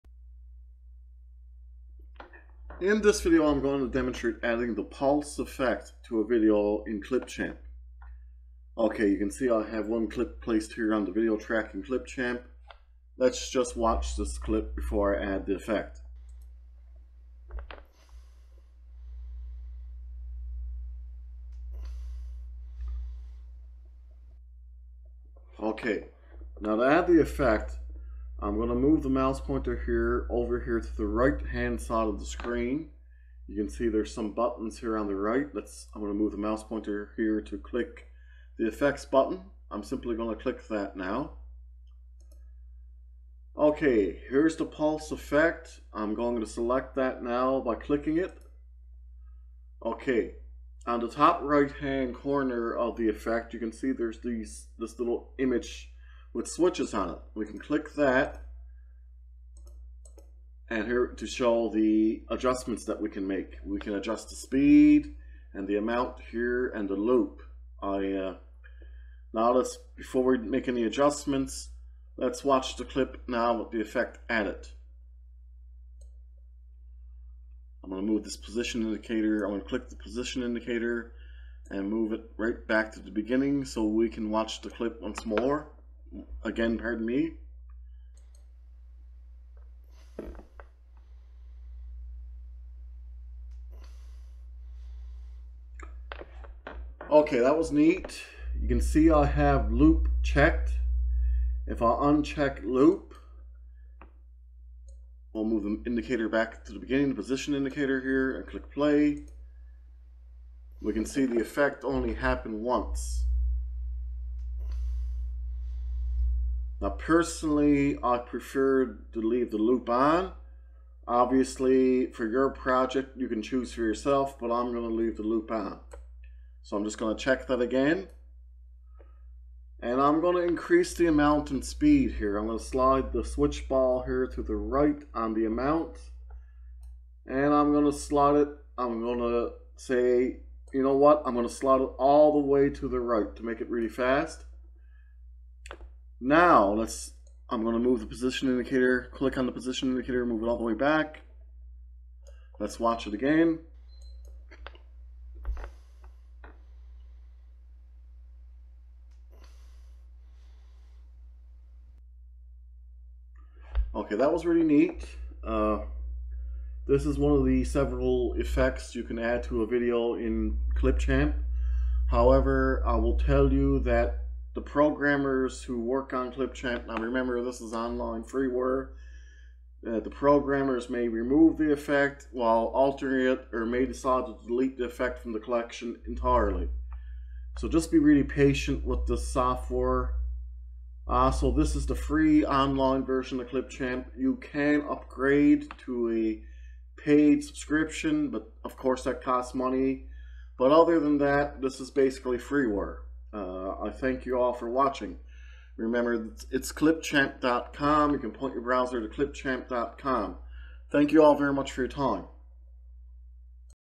Add The Pulse Effect In Clipchamp
In this video we demonstrate adding the pulse effect in Clipchamp.

We used Movavi Screen Recorder to record this video. You can purchase Movavi Screen Recorder here: